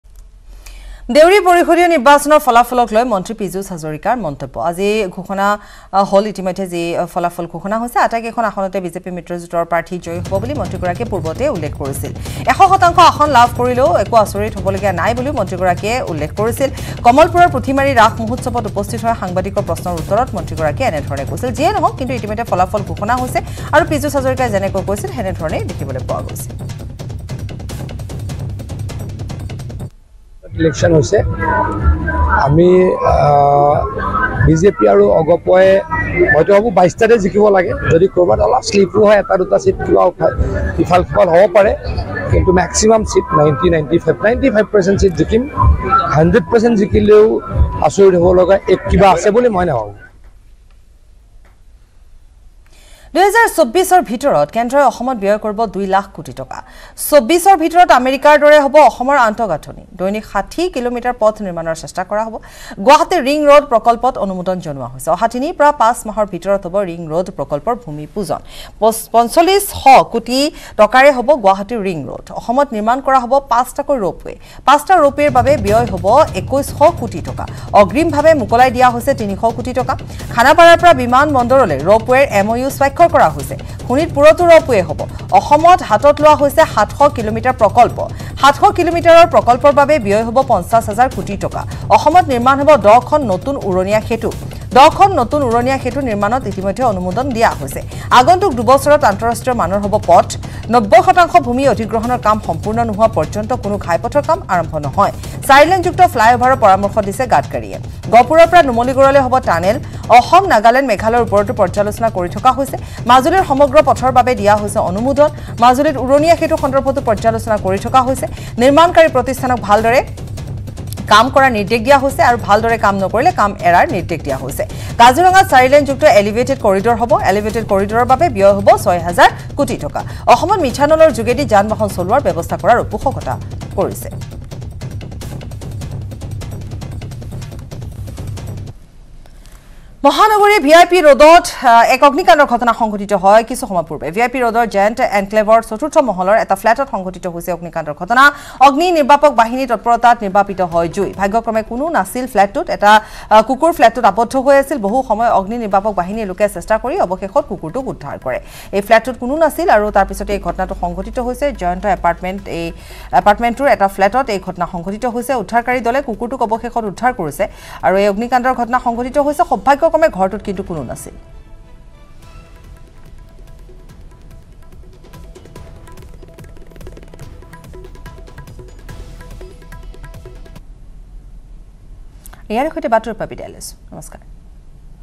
Pijush Hazarika News: Deori Autonomous Councilত ১০০% জয়ী হ'লেও আচৰিত হ'ব লগীয়া একো নাই
Pijush Hazarika News: আজি ঘোষণা হ'ব  Deori Autonomous Council Electionৰ ফলাফল ঘোষণা । লগতে চাওঁক দিনটোৰ বহু গুৰুত্বপূৰ্ণ খবৰ |